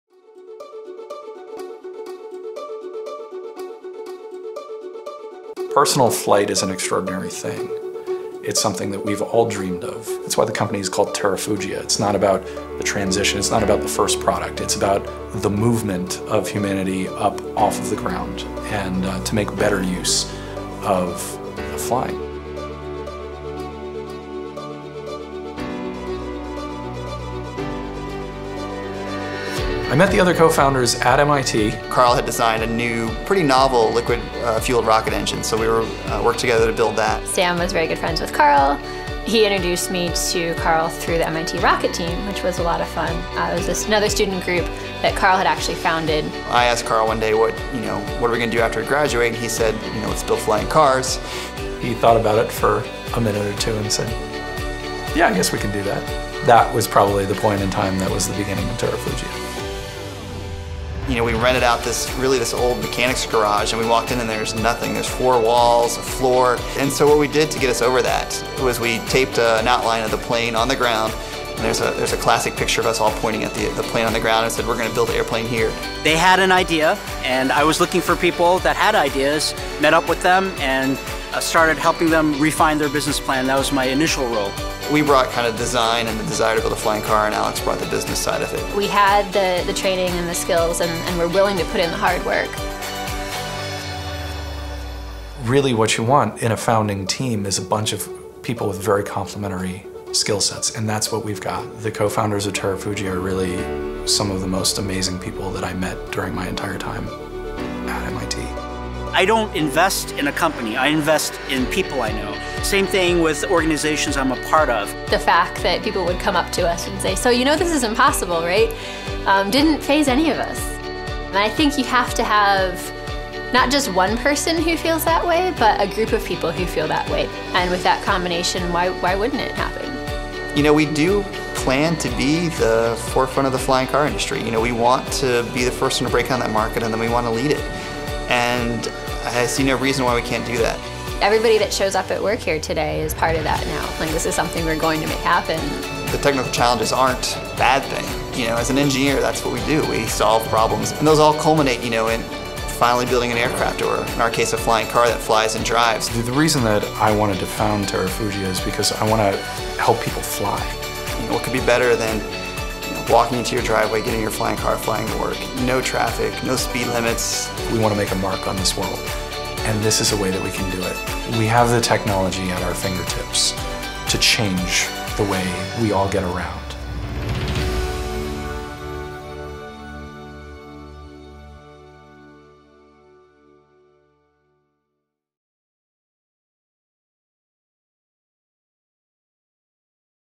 The Founding Terrafugia Team
How did Terrafugia begin?

What drove focused graduate students to make the leap into the unknown?

Listen to Terrafugia Co-Founders Carl Dietrich, Samuel Schweighart, Anna Mracek Dietrich, and Alex Min as they share what drove them to not only create a new company, but define an emerging industry.